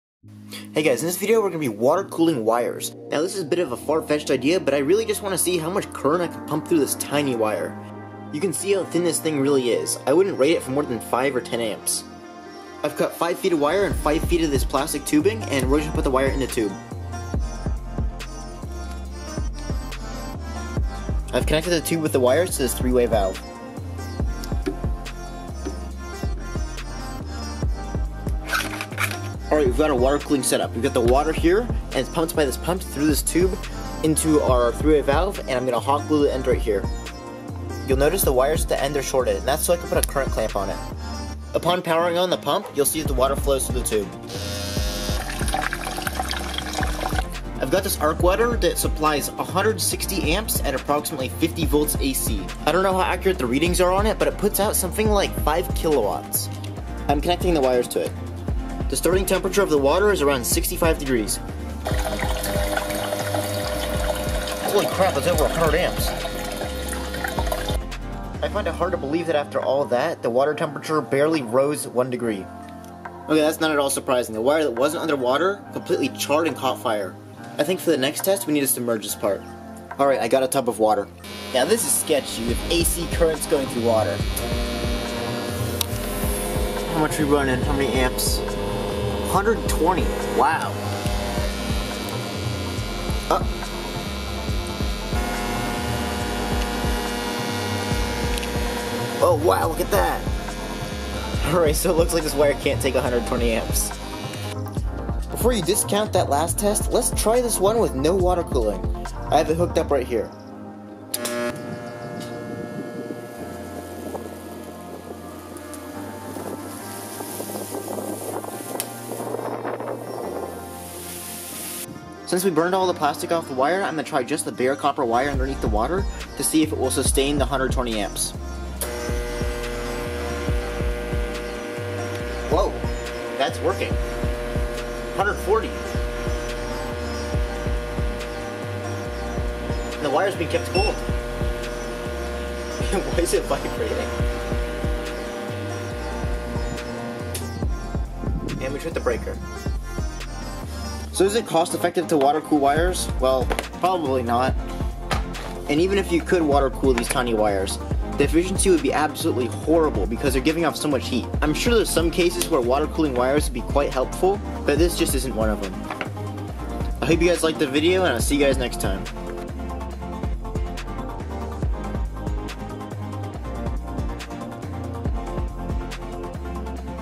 Water Cooling Wires under high load 150+ amps
wanted to see how much power I could continuously push through some thin wire